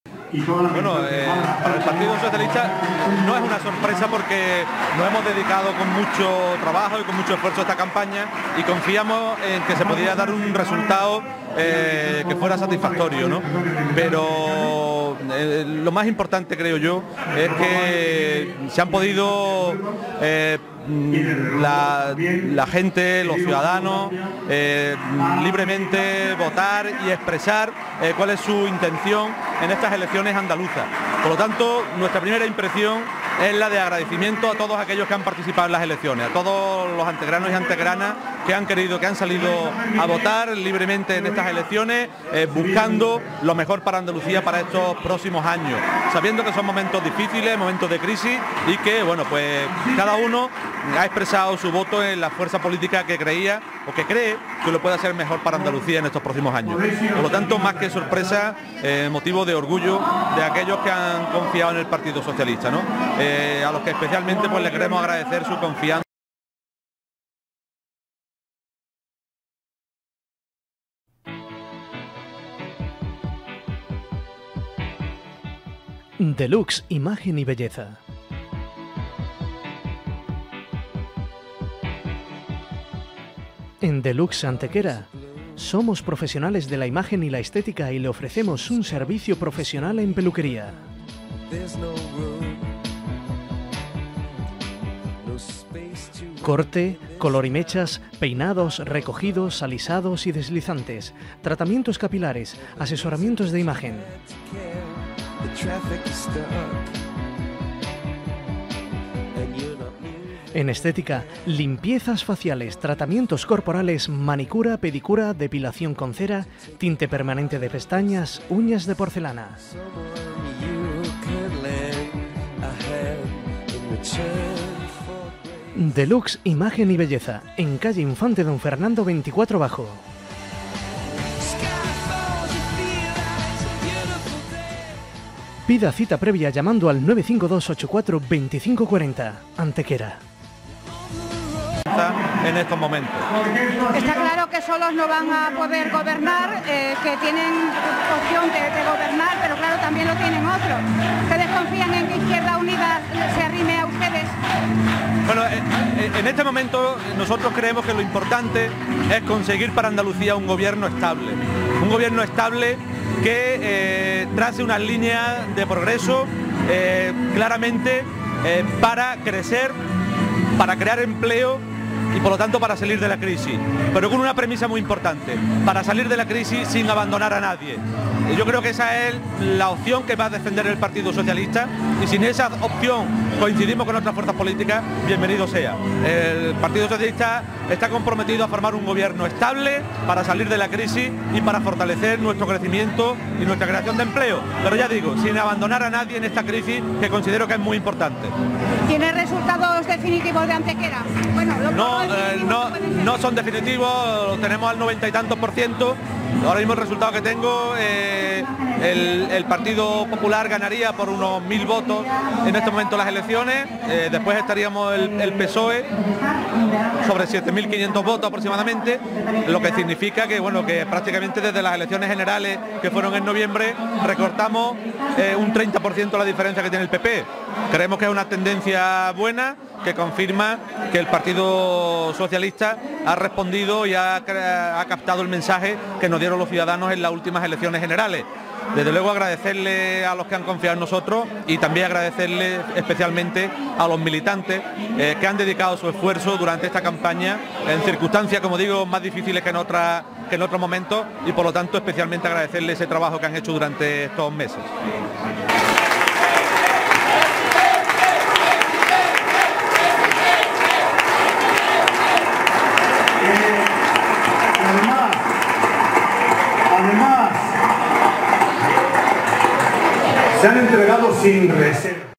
Elecciones Autonómicas, Partido Socialista de Antequera
ATQ Radio-Televisión graba para El Sol de Antequera las declaraciones del concejal socialista José Luis Ruiz Espejo tras los resultados de las elecciones del 25M.